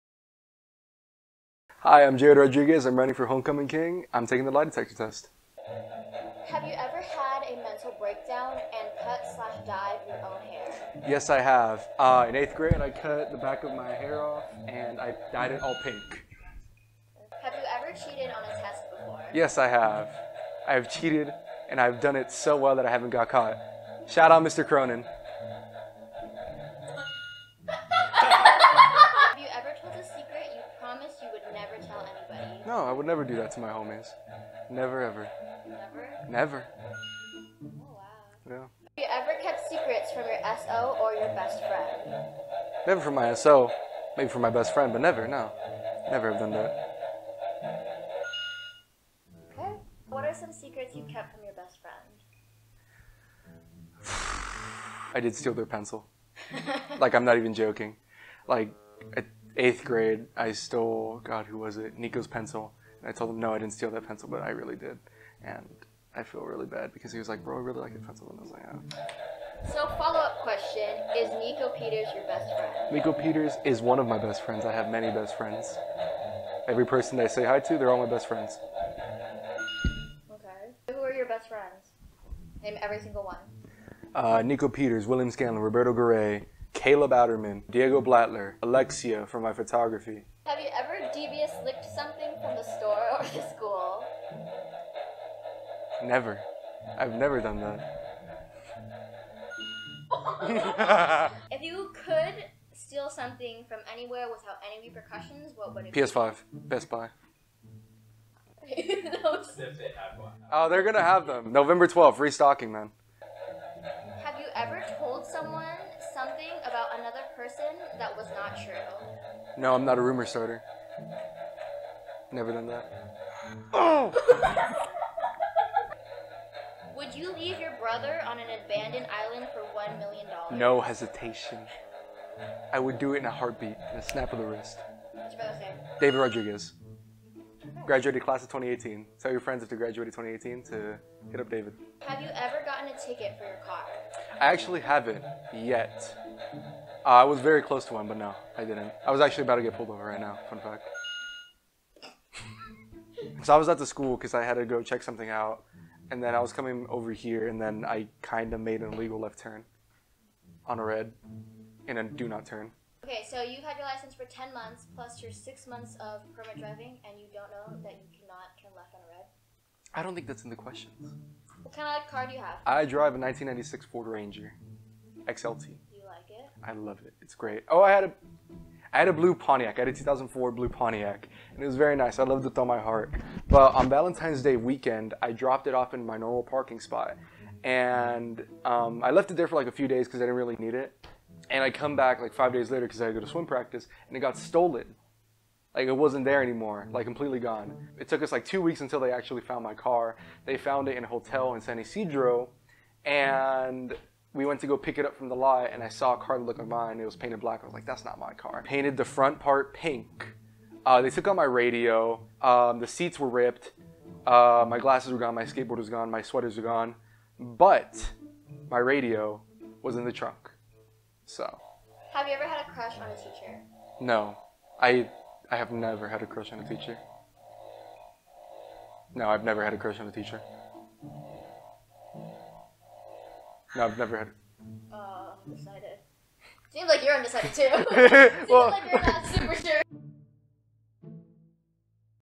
Jared Rodriguez's Lie Detector Test | HOCO 2021
Prince Jared Rodriguez takes a lie detector test, sharing some stories and exposing himself along the way.